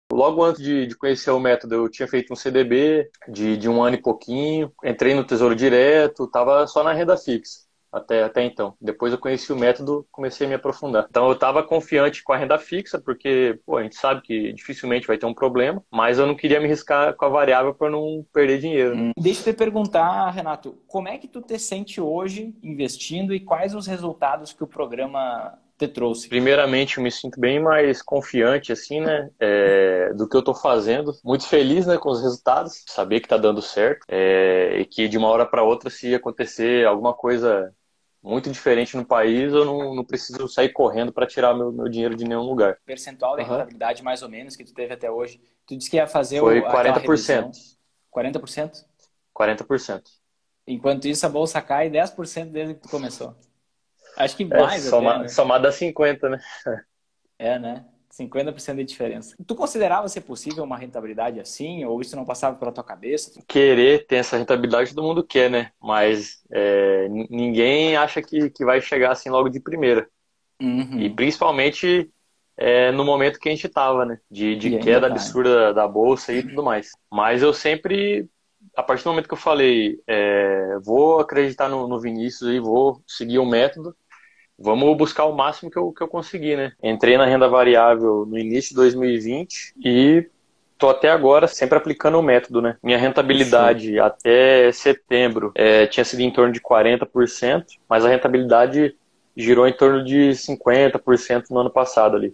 ELE COMEÇOU NA BOLSA NO "PIOR MOMENTO" POSSÍVEL, EM 2020. E MESMO COM AS QUEDAS, LUCROU 50% NO ANO
Ele começou na Bolsa no "pior momento" que poderia poderia existir, no início de 2020. Mas independente das quedas, teve mais de 50% de ganho no ano passado e continua Sempre em Alta.